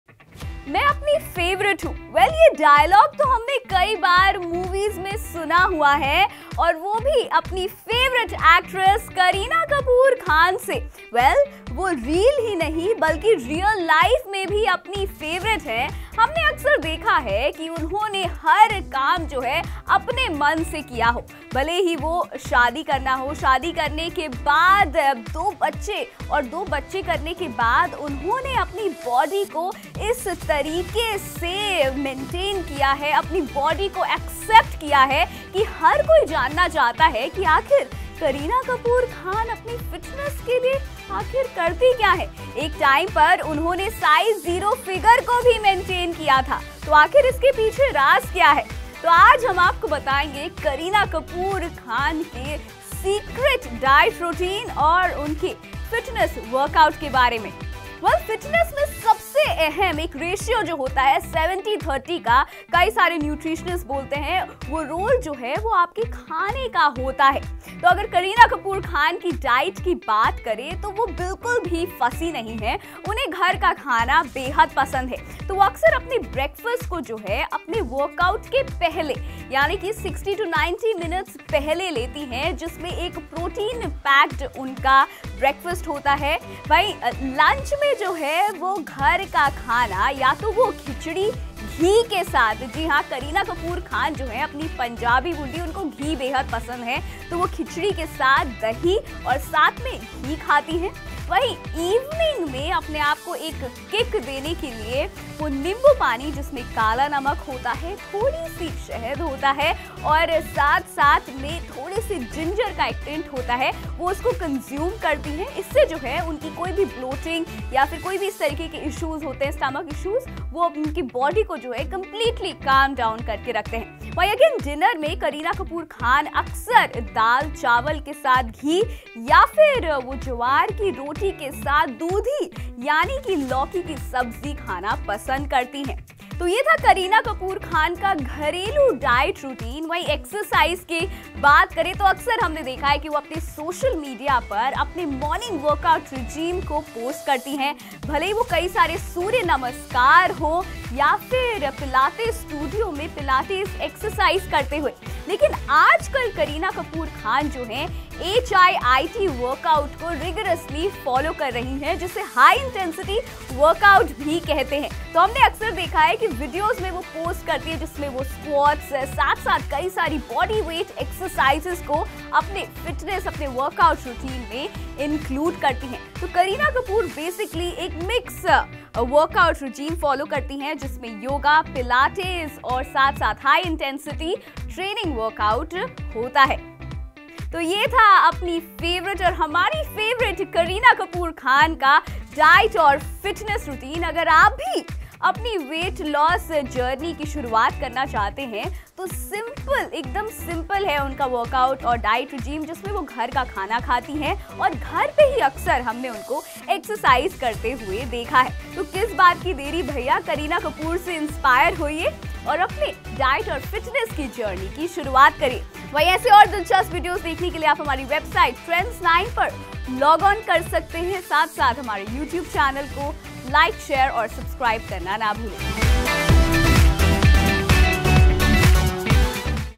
What does Kareena Kapoor Khan eat in a day? Kareen Diet and Fitness Routine Revealed | Celeb Fitness
Are you curious about what Kareena Kapoor Khan eats in a day? Well, in this video, we're uncovering her secret diet and fitness routine! Kareena Kapoor Khan is one of the most popular and well-known celebrities in the world, and her diet and fitness routine is a mystery to most people. In this video, we're revealing her secret diet and fitness routine, and you won't believe what she eats! From healthy breakfast foods to healthy snacks, Kareena Kapoor Khan's diet and fitness routine is a secret you won't want to miss!